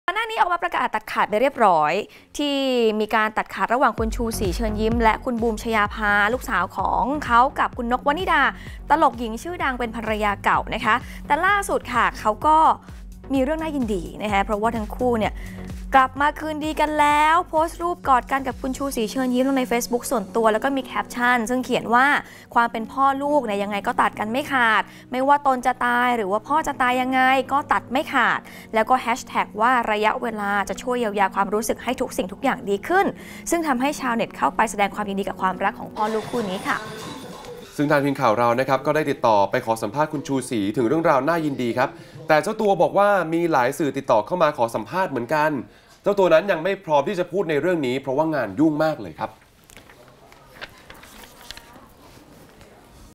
"ชูษี เชิญยิ้ม" โผกอด คืนดี ลูกสาว
หลังจากเคยมีเรื่องมีราวกันจน ชูษี เชิญยิ้ม ออกมาประกาศตัดขาดความเป็นพ่อลูกกันกับ บูม ชญาภา ลูกสาวของเขา แต่ล่าสุดมีเรื่องให้น่ายินดีเมื่อ บูม ชญาภา ได้โพสต์รูปกอดกับ ชูษี เชิญยิ้ม ลงในเฟซบุ๊กส่วนตัวพร้อมแคปชั่นว่า ความเป็นพ่อเป็นลูกยังไงก็ตัดกันไม่ขาด ไม่ว่าตนจะตายหรือพ่อจะตายยังไงก็ตัดกันไม่ขาด